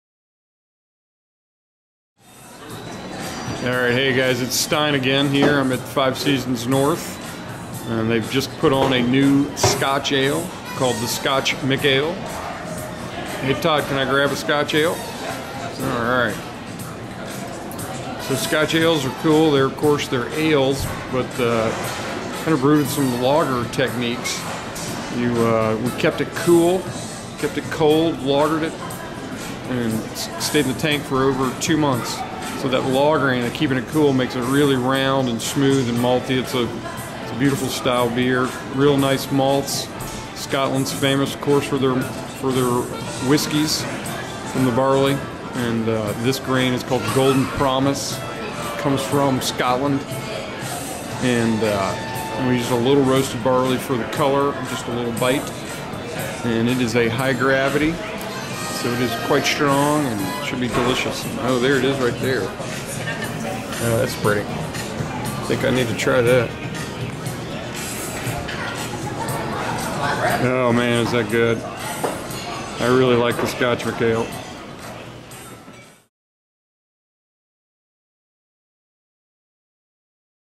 Stein does the new Scotch McAle
Stein tries the new Scotch McAle.